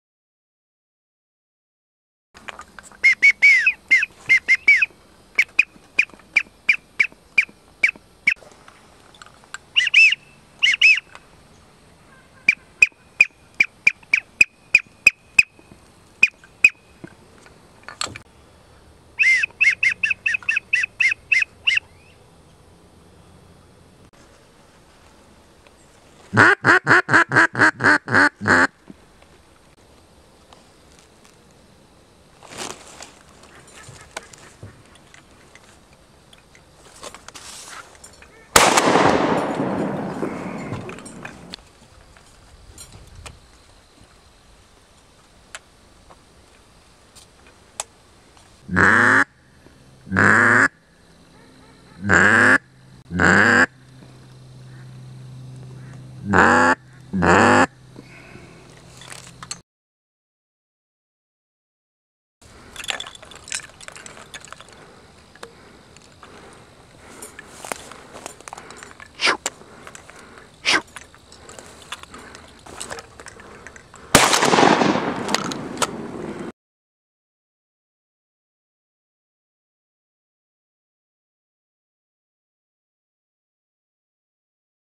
Early Teal and Goose Hunt LaSalle FWA, Blind 23
The view from Blind 23. A few birds flew today and couple got educated.

Much of my work is for sale. If you are interested in owning some of my work, just message me or visit my pages at:

Thanks for the view and comments are welcome.